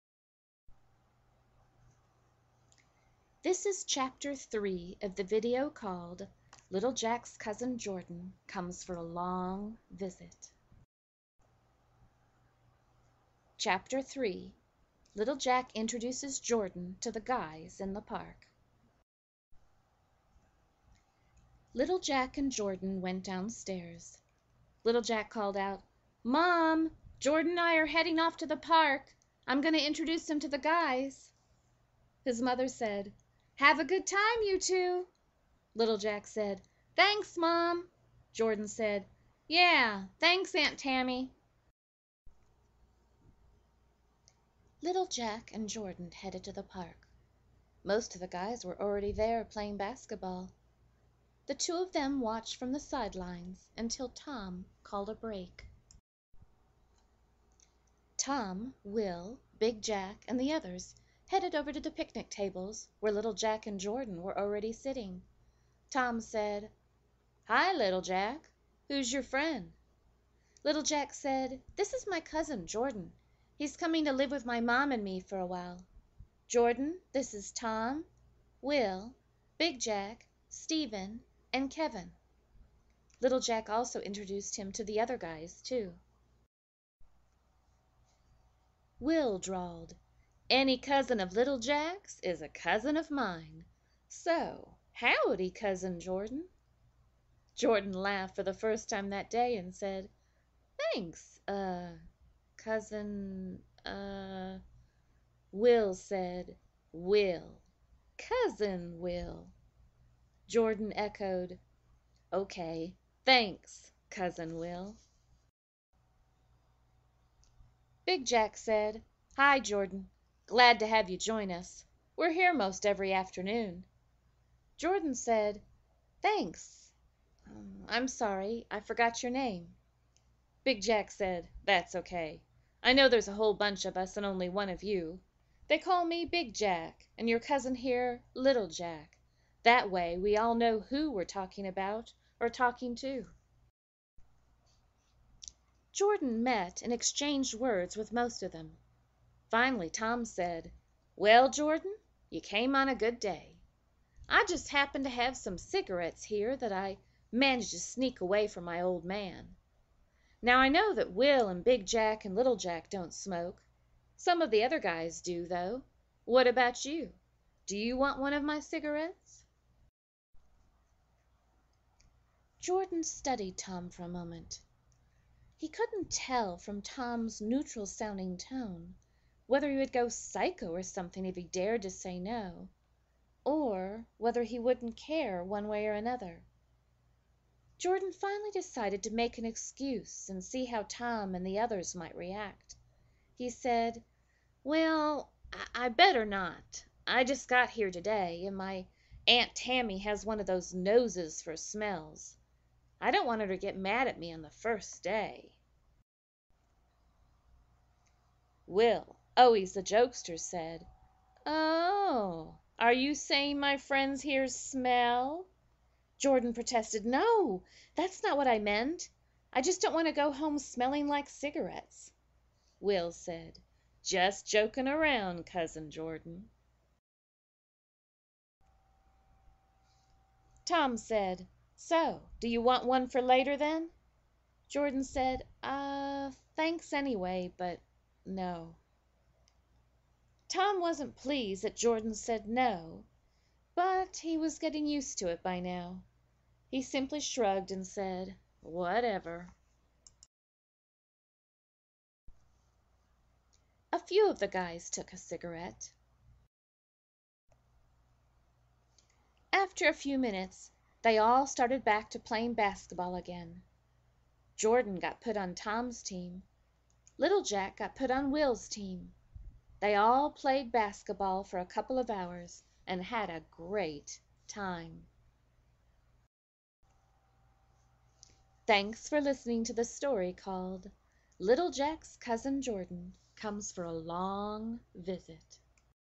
Video -- Chapter 3 of 3: Little Jack's cousin Jordan comes for a long visit
In the YouTube video, the author dictates chapter 3 of the sixth of eight stories about Little Jack's hero's journey. It is called "Little Jack's cousin Jordan comes for a long visit."